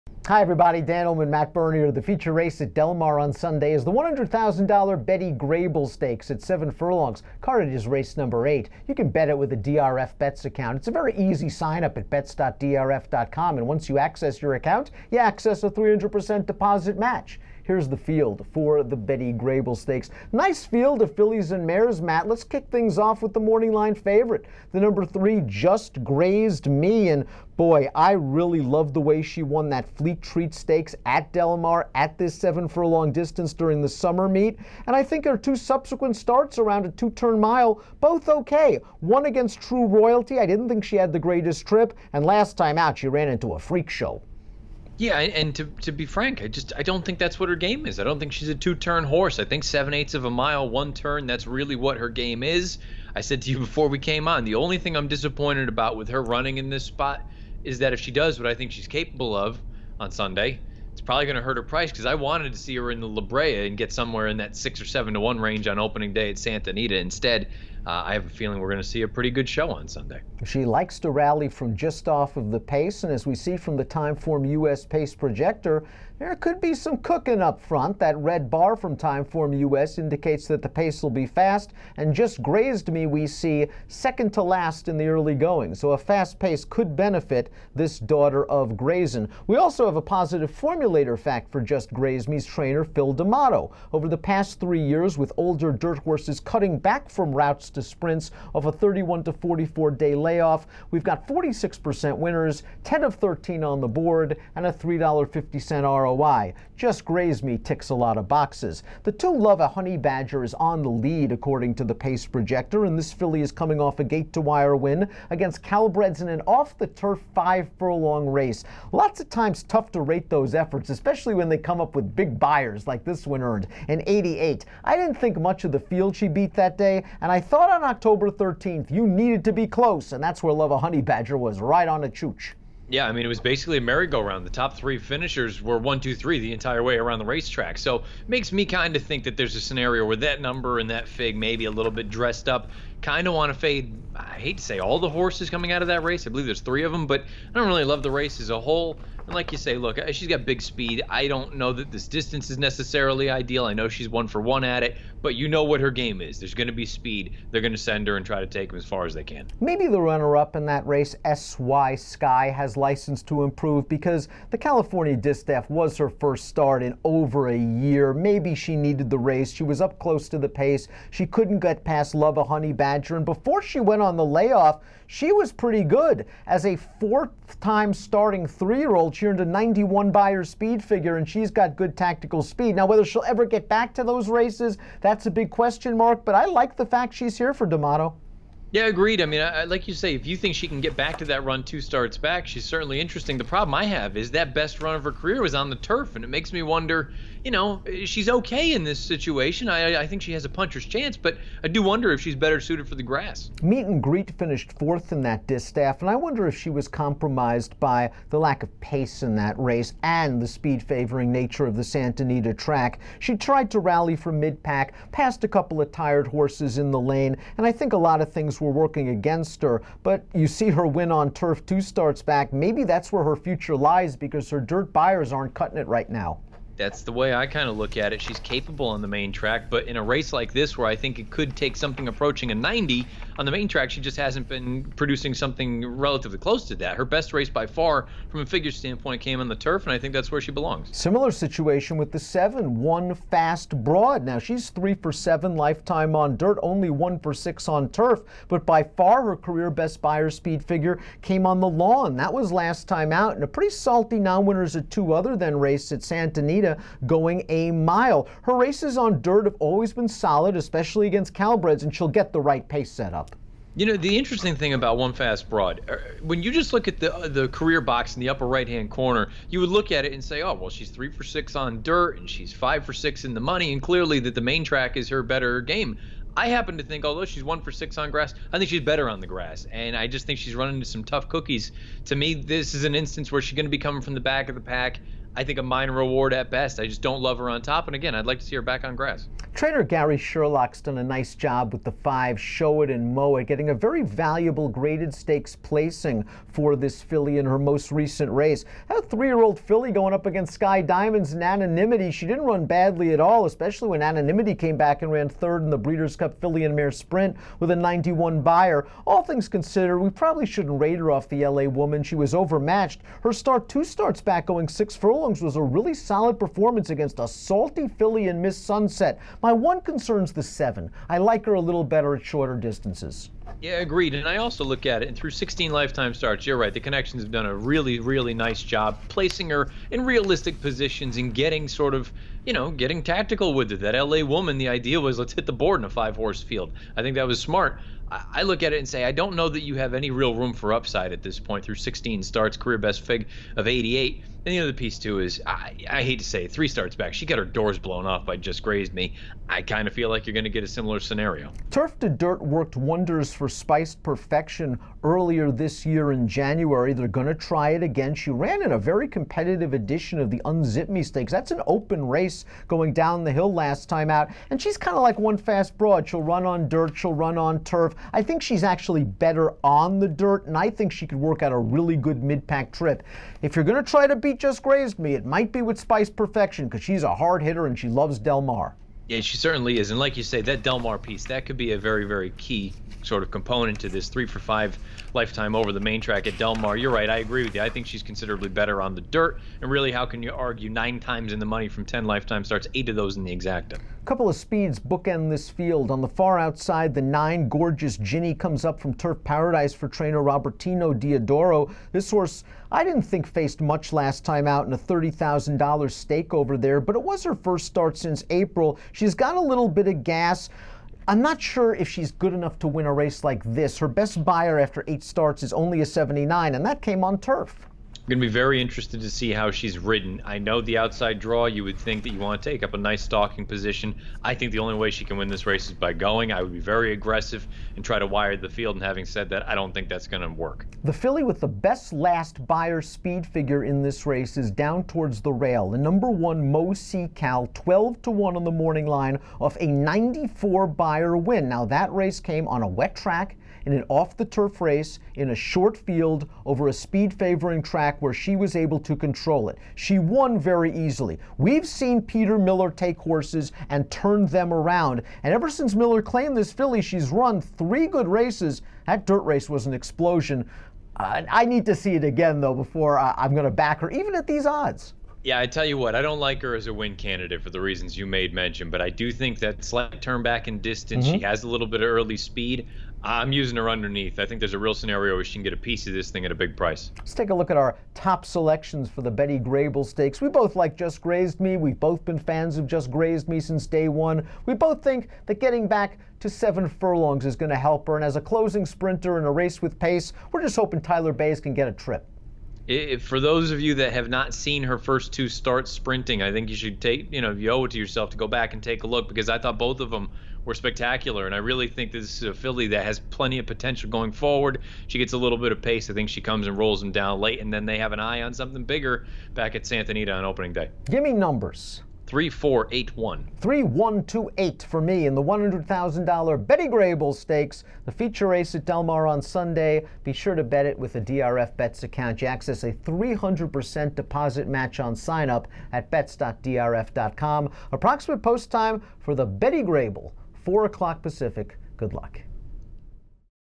Betty Grable Stakes Preview 2018
Dan Illman and Matt Bernier take a look at the Betty Grable Stakes at Del Mar